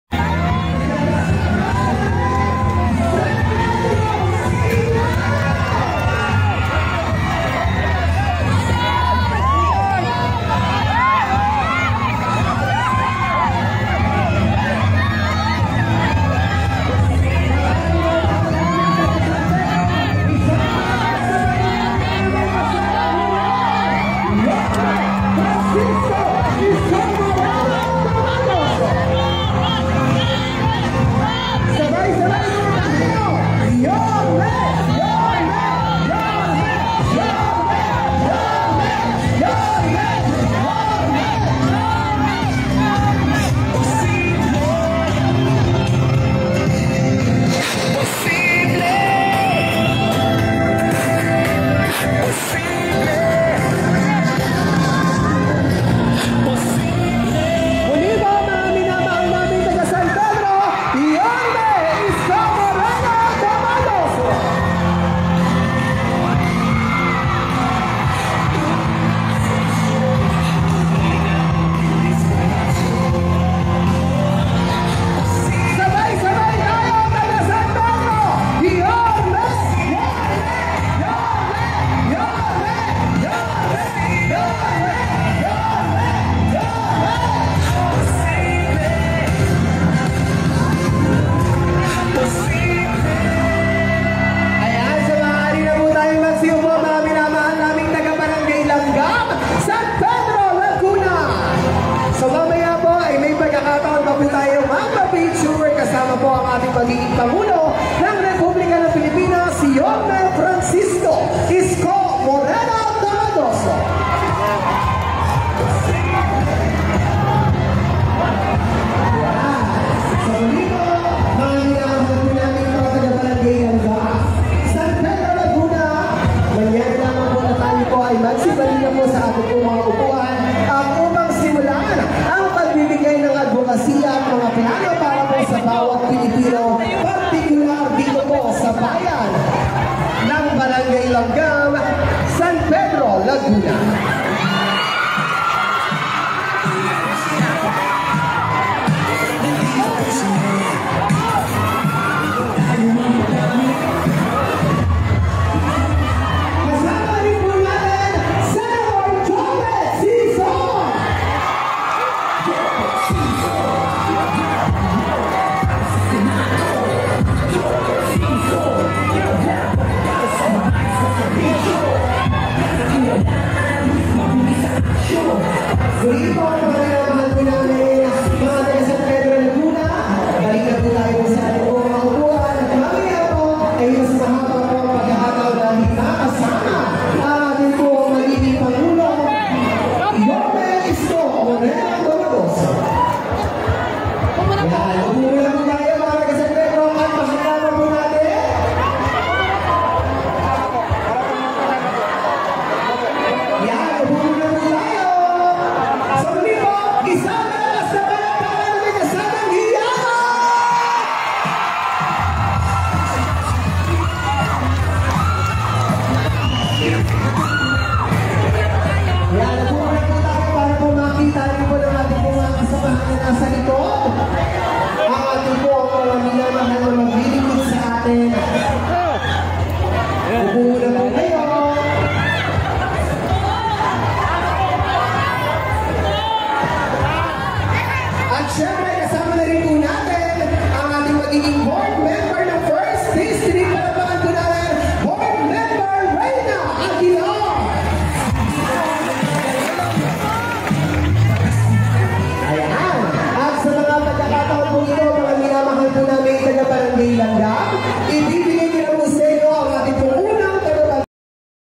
Mayor Isko Moreno. Pinag Kagulohan sa San Pedro Laguna
Dami tao sa laguna Yun ang wla hakot at wla bus legit na supporter

This video is Edited under by Fair use of YouTube

This video Courtesy of Bro John Castriciones.